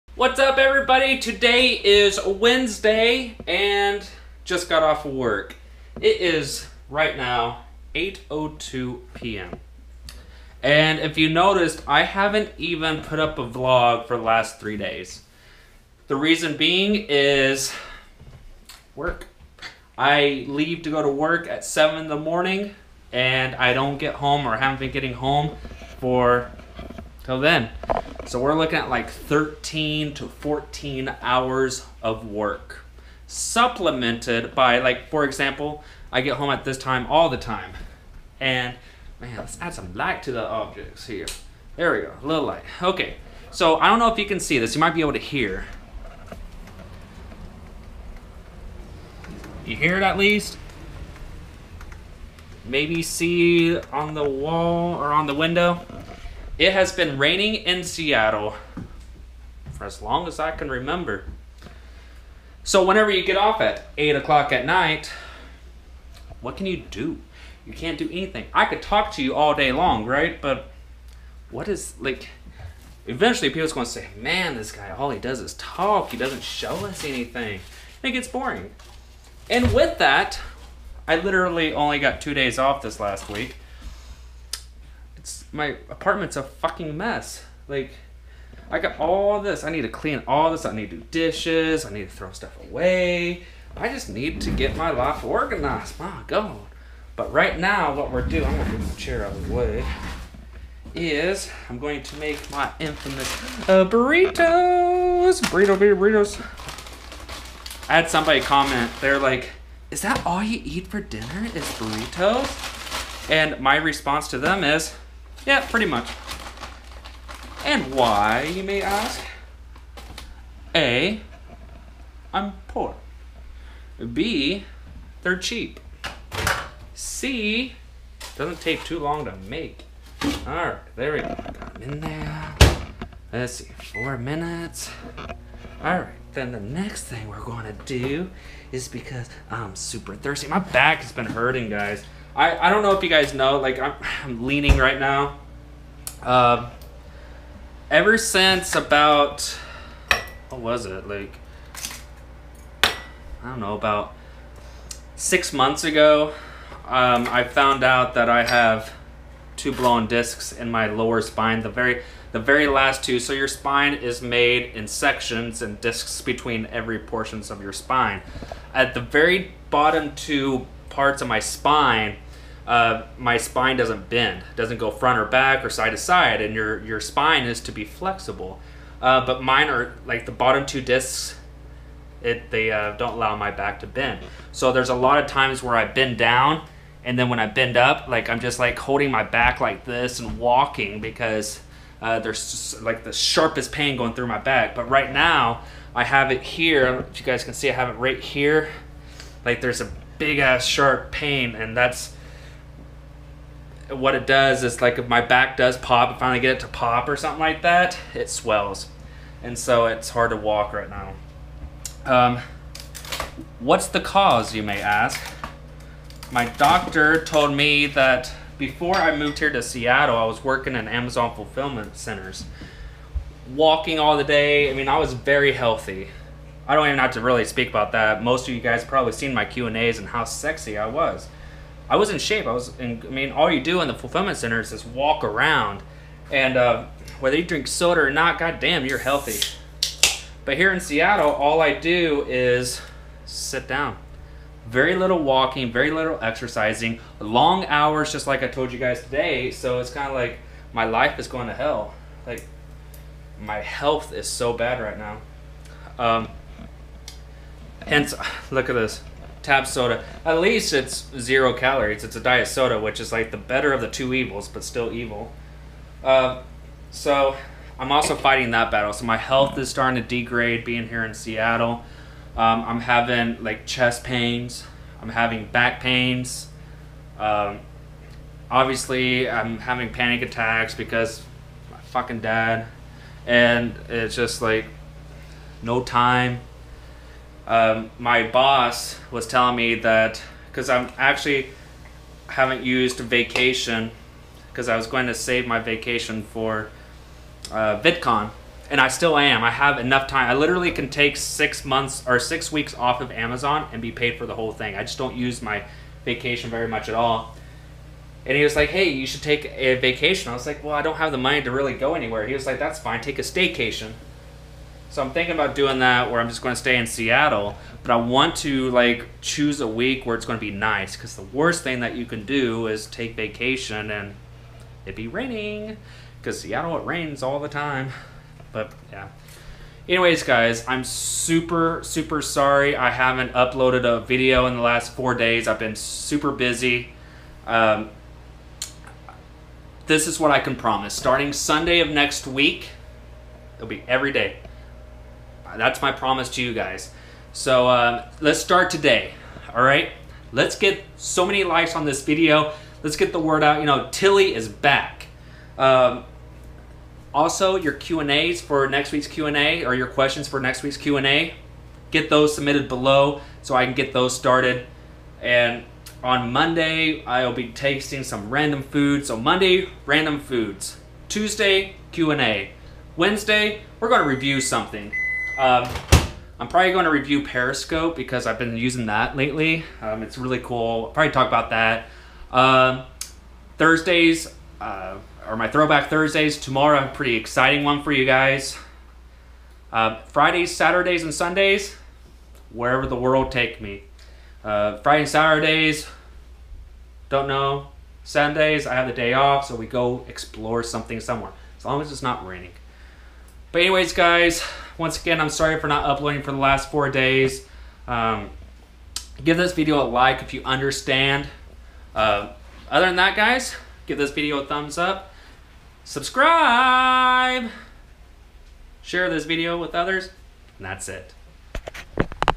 A Vlog full of Nothingness
Today I get off work late and I explain why I haven't posted many VLOGs this week

Send Fan Mail to the Address Below

TilleyVision Studios LLC
ATTN: Fan Mail
1806 12th Ave. B202